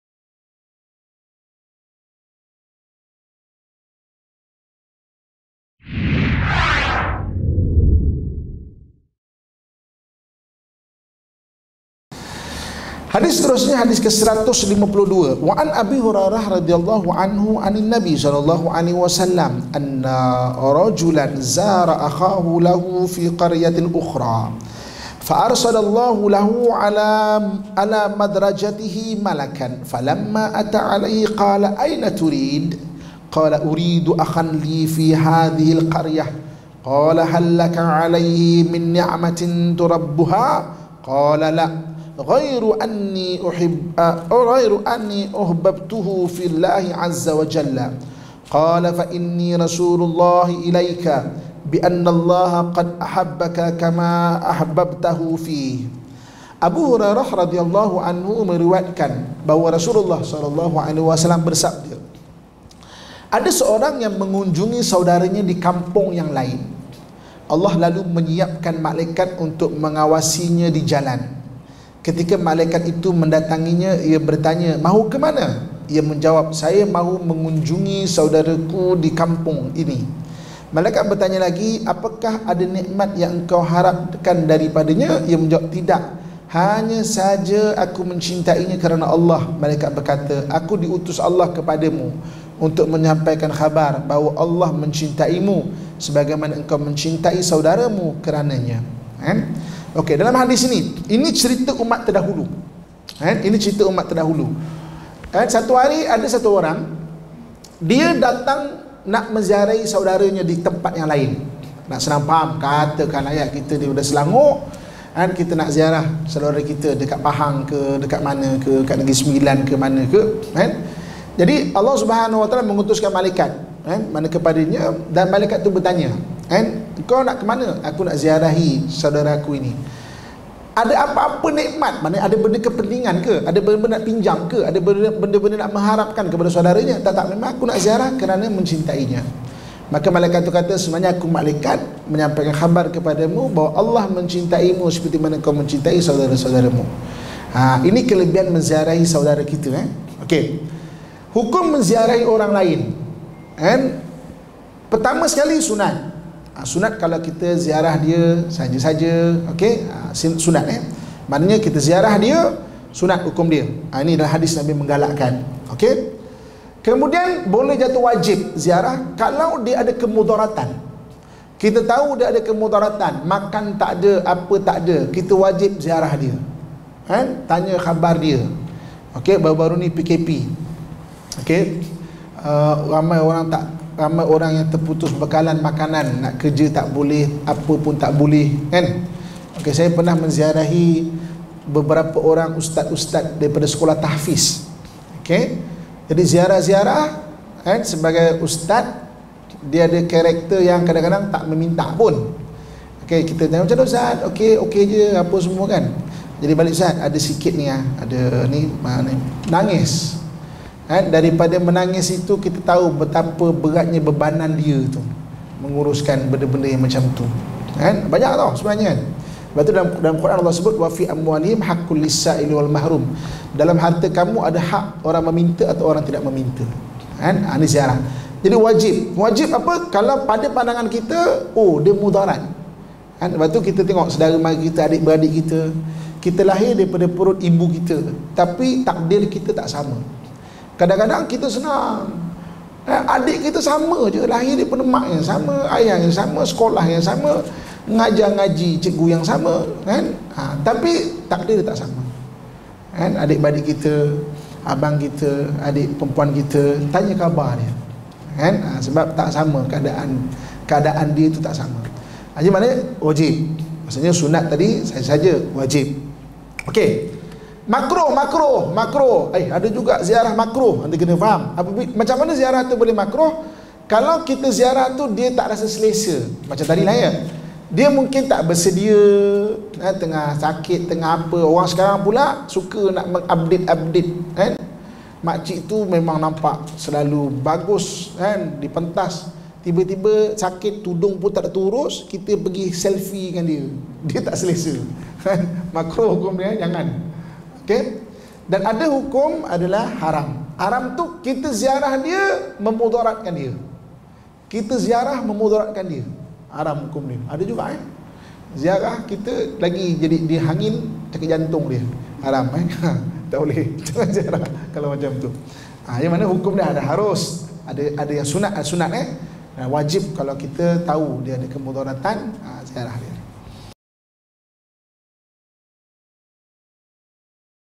Ustaz Amin - Hadith Mukhtar 152
Kuliah Berkitab Mukhtar Fadhoil Amal bermaksud Himpunan Hadith Kelebihan Amalan yang telah disusun oleh Al Fadhil Ustaz Muhamad bin Abdullah Al Amin sebanyak 245 buah Hadith soheh. Mengandungi 10 bab iaitu Keutamaan Tauhid, Iman, Ilmu, Dakwah, Jihad, Ibadah. Didalam ibadah pula ada pecahan seperti Taharah, Azan, Solat, Hari Jumaat, Solat Sunat, Sedekah, Pinjaman, Memberi Air, Mengolah Tanah & Bercucuk Tanam, Puasa, Haji & Umrah, Zikir & Selawat & Doa. Pada bab 8 Keutamaan Dalam Bab Pergaulan. Semoga kita semua mendapat manfaat dan kebaikan untuk kita semua inshaallah amin ~ ADMIN